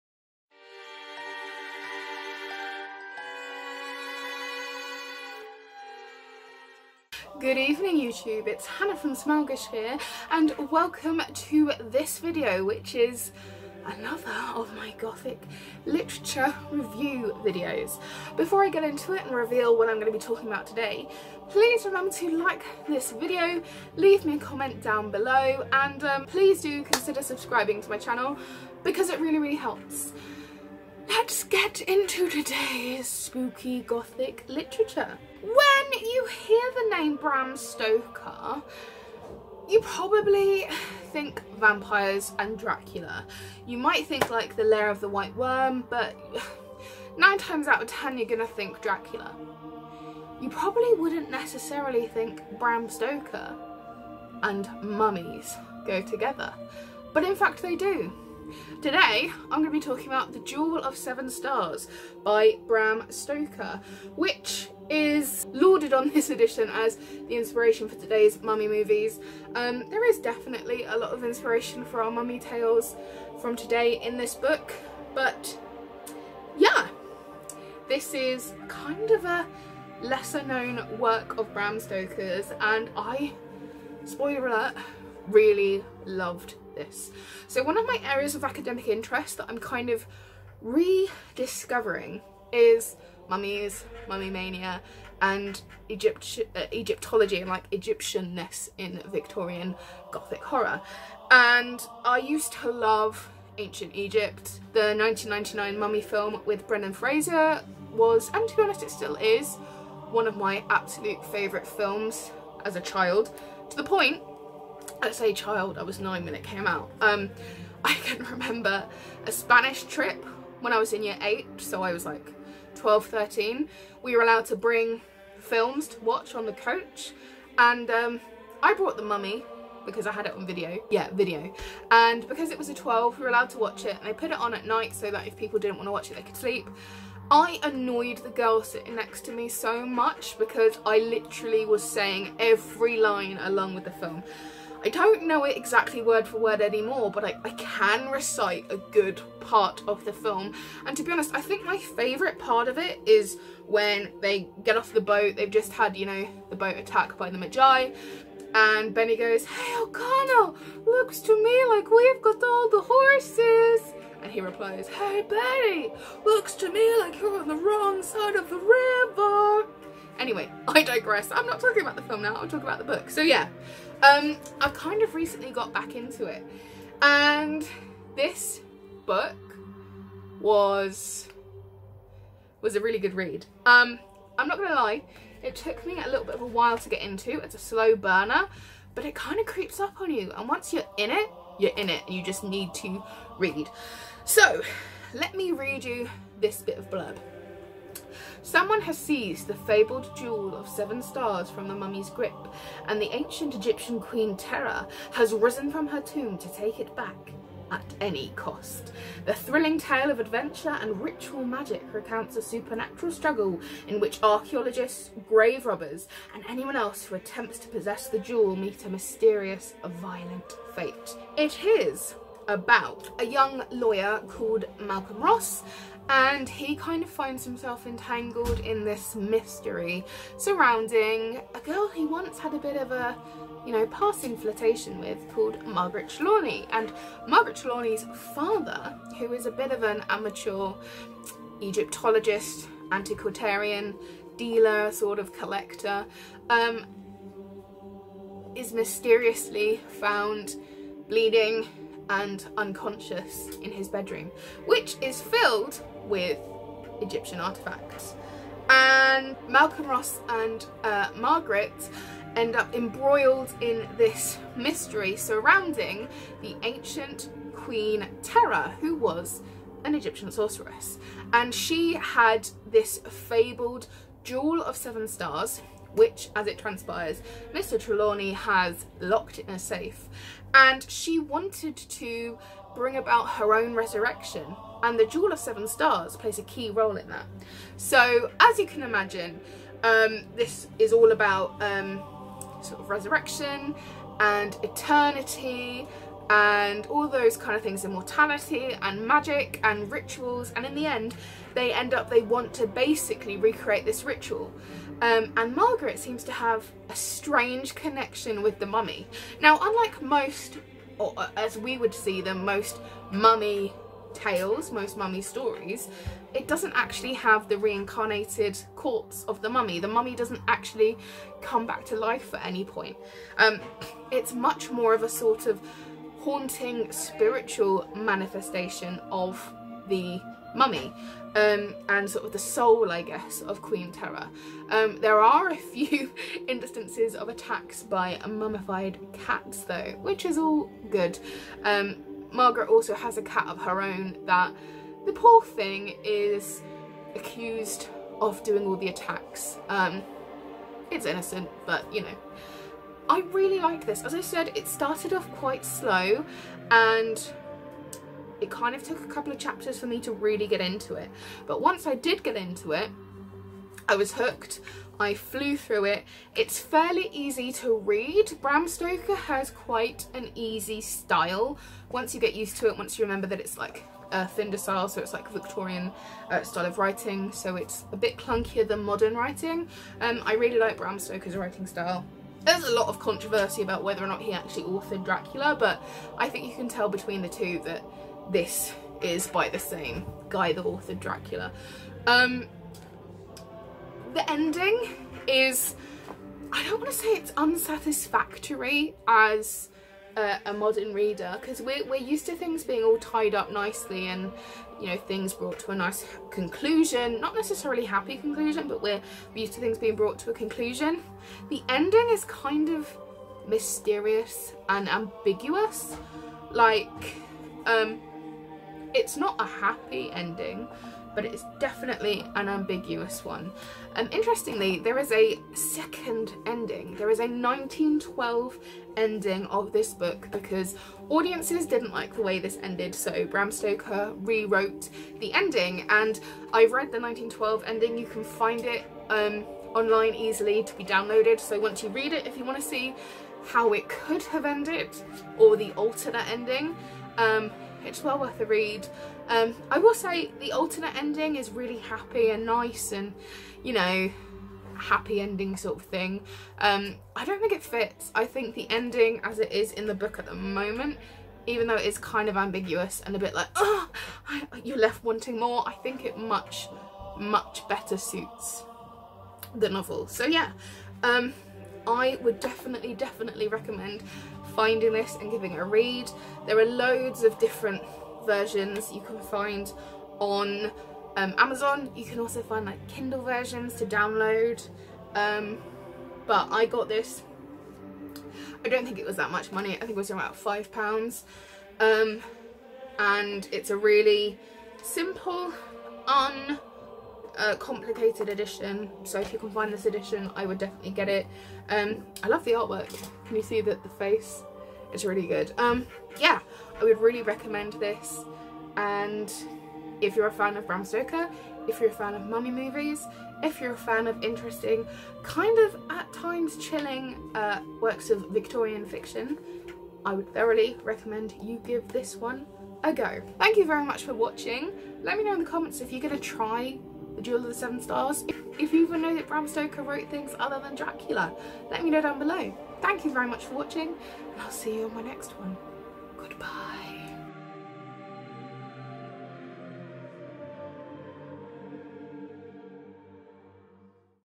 THE JEWEL OF SEVEN STARS by BRAM STOKER | Gothic Literature Series #4 Smaugish
Good evening lovely spookies!

Today I am bringing you another of my Gothic Literature review series. This time, one of Bram Stoker's lesser known works- and his venture into Egyptology and the mummy mythos- The Jewel of Seven Stars. I really enjoyed this one, so keep on watching to find out more!